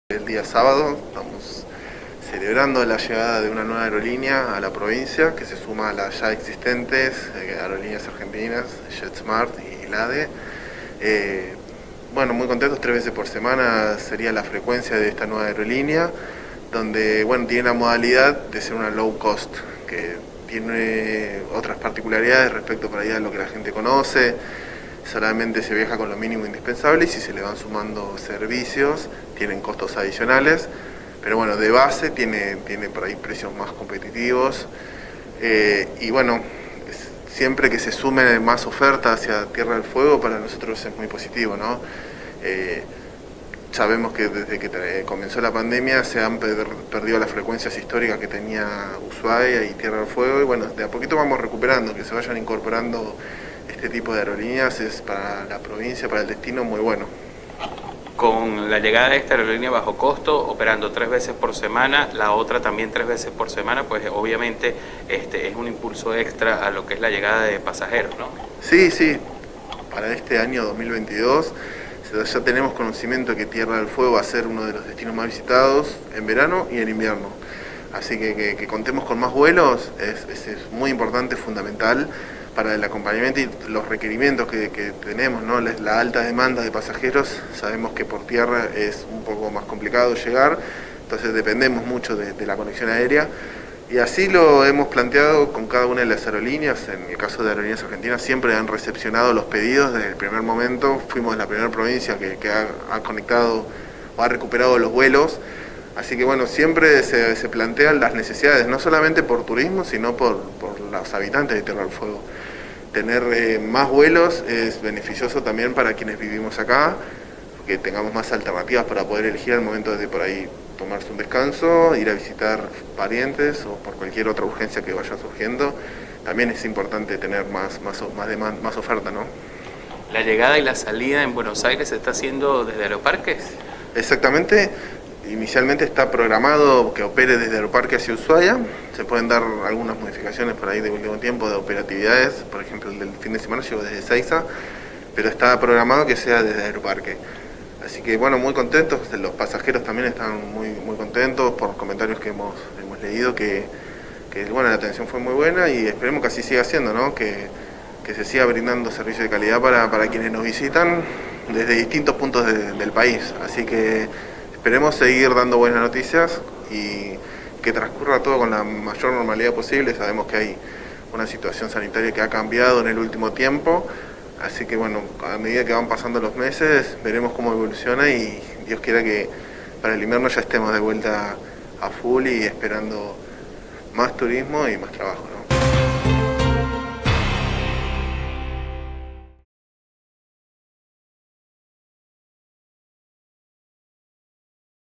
NUEVA AEROLÍNEA BAJO COSTO LLEGÓ A USHUAIA.
Pavlov comunicó que la empresa es Flybondi y tendrá 3 días de vuelos a la ciudad durante la semana